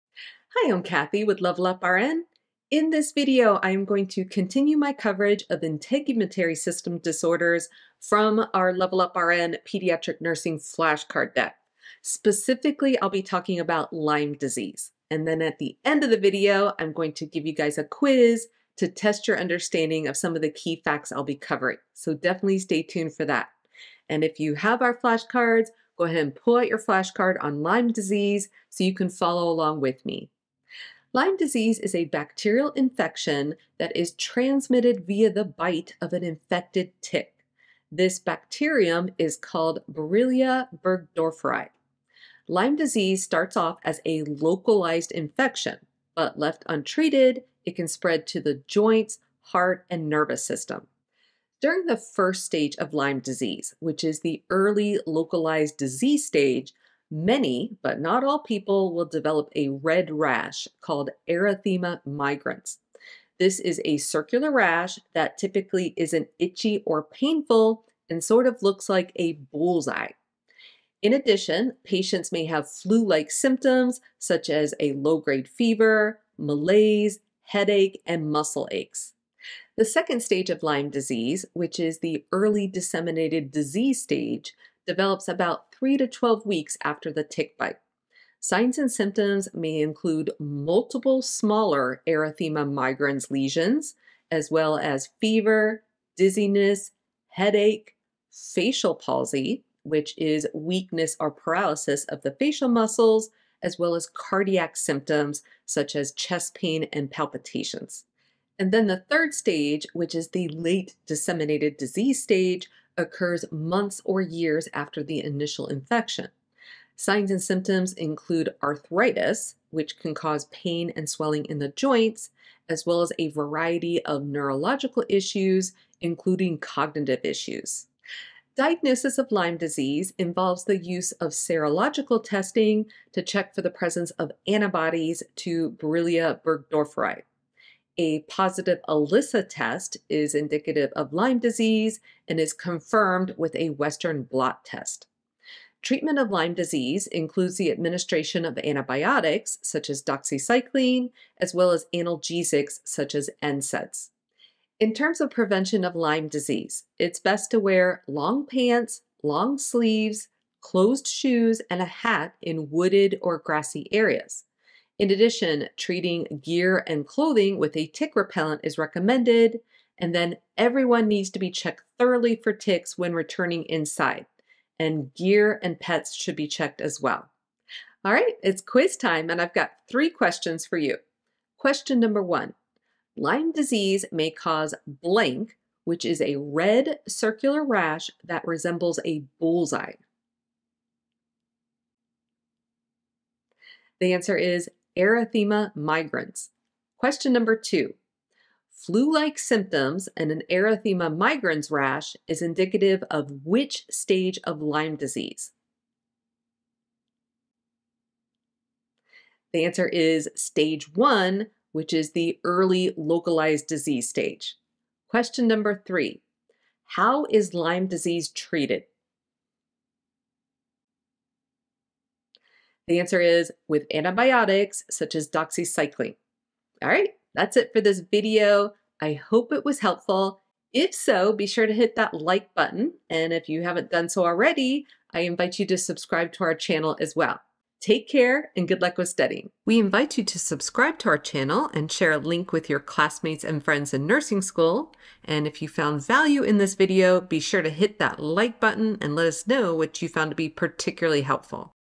Lyme Disease: Alterations in Health - Pediatric Nursing, Integumentary System
Cathy discusses Lyme disease. She explains the pathophysiology, signs and symptoms for each stage of lyme disease, diagnosis of lyme disease, treatment of lyme disease, and prevention of lyme disease. At the end of the video, Cathy provides a quiz to test your understanding of key facts she covered in the video.

00:00 Introduction
00:30 Lyme Disease
2:57 Quiz Time!